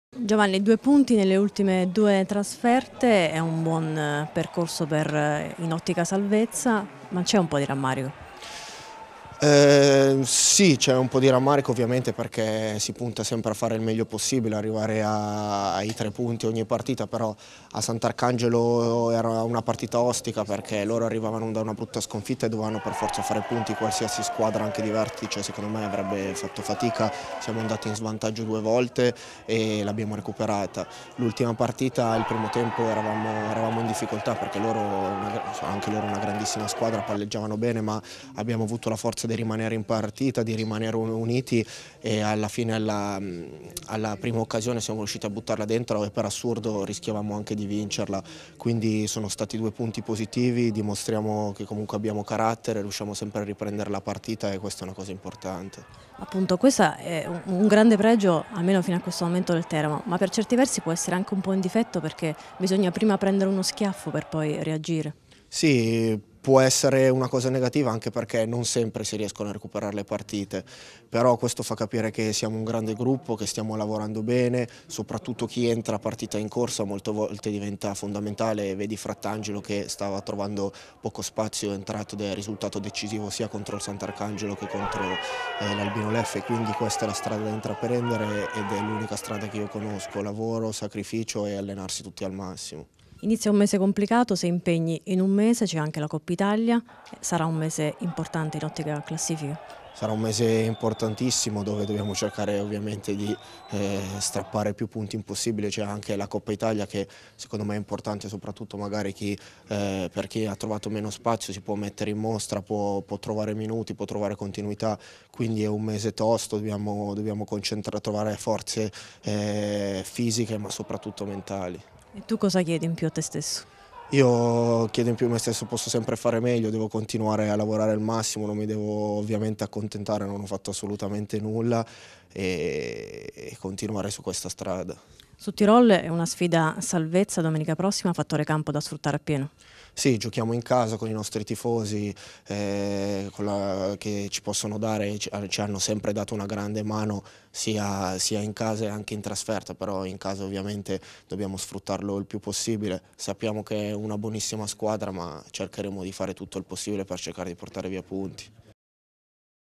Graziano: “C'è un po' di rammarico per i due pareggi”
Tornato titolare a Bergamo, il centrocampista del Teramo, Giovanni Graziano parla dei due ultimi pareggi dei biancorossi in trasferta e mette nel mirino un mese, quello di novembre, che sarà fitto di impegni per il Diavolo